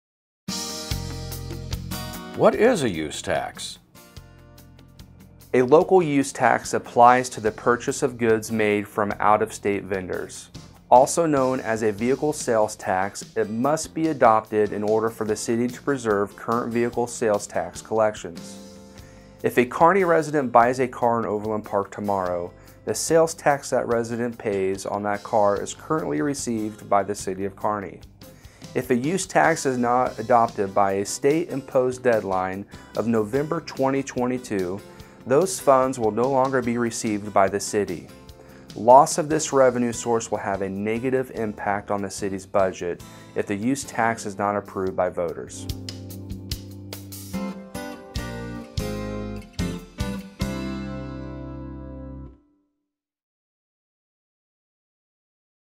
What is a use tax?
A local Use Tax applies to purchase of goods made from out-of-state vendors. Also known as a vehicle sales tax, it must be adopted in order for the City to preserve current vehicle sales tax collections. If a Kearney resident buys a car in Overland Park tomorrow, the sales tax that you pay on that car is currently received by the City of Kearney. If a use tax is not adopted by a state-imposed deadline of Nov. 2022, those funds will no longer be received by the City. Loss of this revenue source will have a negative impact on the City’s budget, if the Use Tax is not approved by voters.